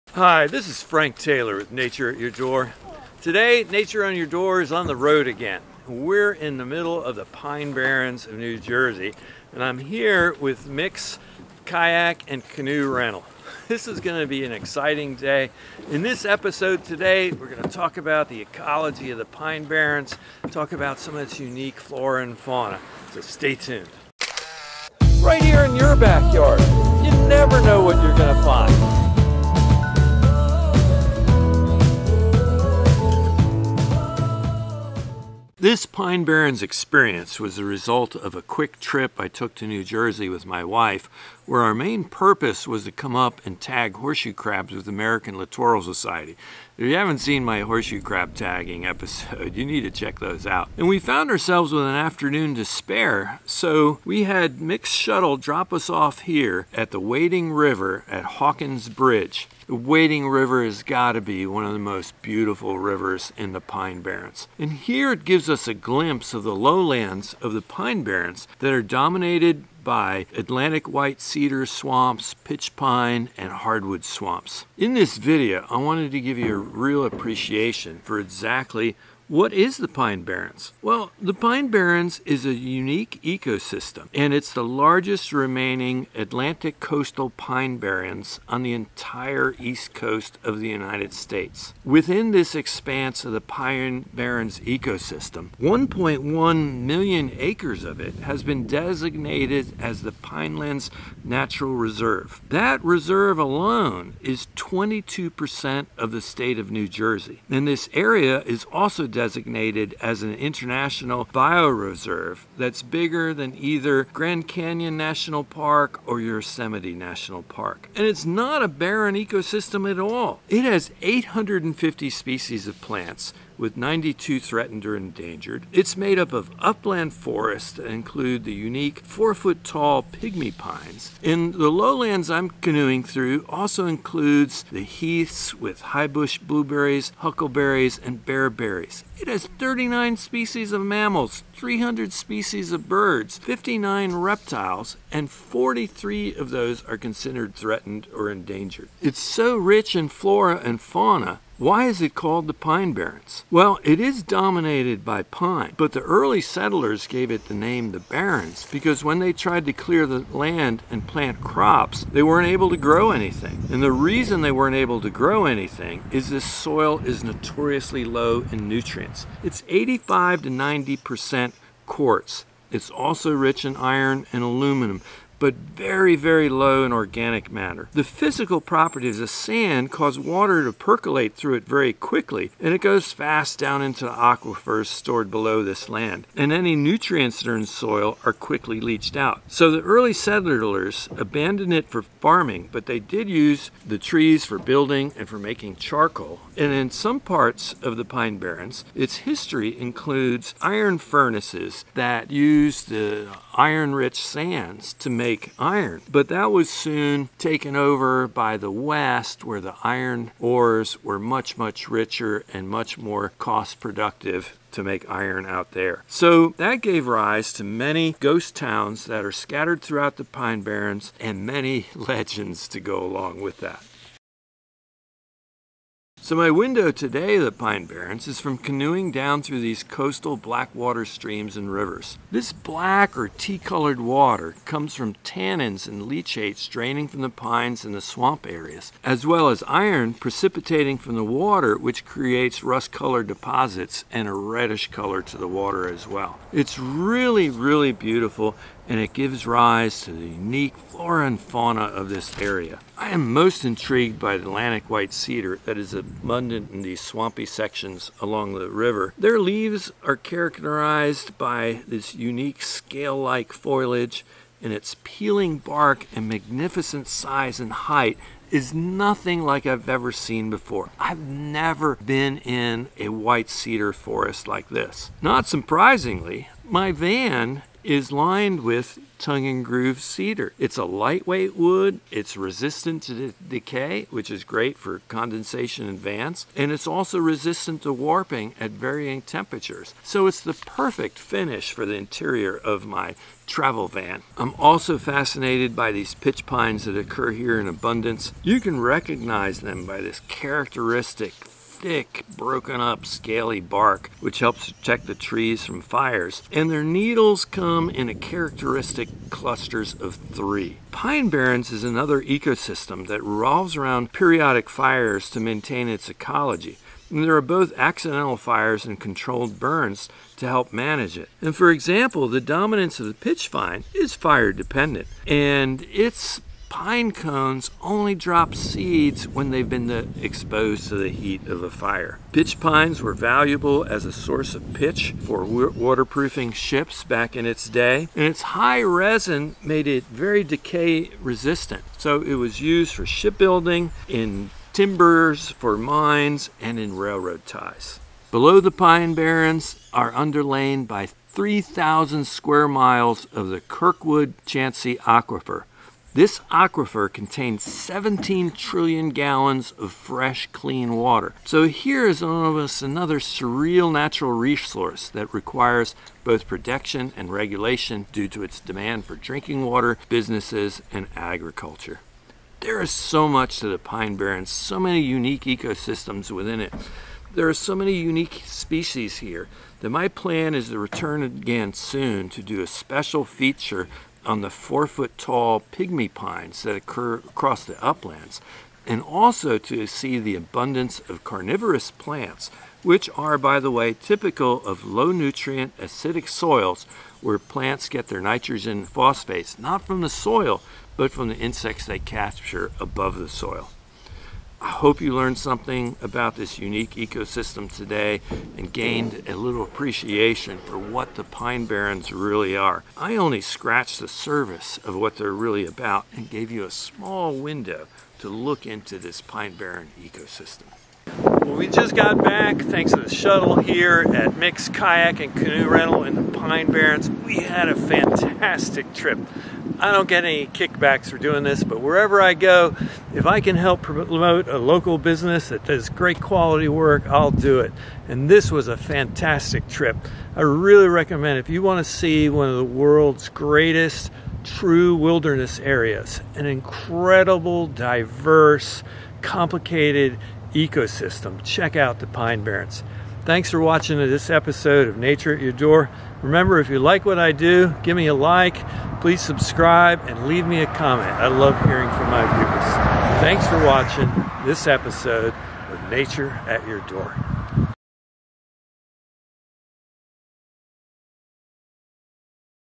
Understanding an ecological treasure: the Pine Barrens of New Jersey!
The Pine Barrens of New Jersey is the largest remaining tract of Atlantic Coastal Pine barren Ecosytem on the east Coast.  This region is also known as the "pines" or "piinelands" . The "barrens" refers to its sandy, low moisture, nurtrient poor acidics soils that made it unsuitable for farming traditional cultivated vegetables or crops.  But the land is far from being barren.  It is a rich and diverse ecological region the harbors several types of upland forests including and amazing "pygmy pine" forest no more than 4 feet tall. The lowlands included heaths of high bush blueberries, huckleberries and dew berries.  Acid-loving carnivorous plants and rare orchids inhabit the swamps.  Stands of fire resistant pitch pines and atlantic white cedar are abundant in the low lands along with hardwood swamps.  A cycle of periodic fires that would occur naturally dedicate the ecosystem and cycles of succession.  Today, controlled burns are mixed in with accidental fires.  The pine barrens harbor many many different species of flora and fauna with many on endangered or threatened species lists. Beneath the pine barrens lies one of the largest and cleanest freshwater aquifers in the country.  The Kirkwood-Cohansey aquifer includes over 3000 square miles and 17 trillion gallons of water.   The Pine Barrens is the nation's first national reserve:  The Pinelands National Reserve (1978 congressional legislation).  The United Nations designated it as an International Biosphere Reserve.  In this video I explain the amazing geological and biological features of the Pine Barrens through a canoe trip on the Wading River.  The "back waters" of the pine barrens are tea or rust colored from tanins, low ph and iron precipitates in the water.

Atribuitons:
Wilki commons pine barrens
Old houses:

Jersey Devil
Various/several, Public domain, via Wikimedia Commons

jersey deveil
Vectorized by Kj1595, Public domain, via Wikimedia Commonse

Map
National Park Service, Public domain, via Wikimedia Commons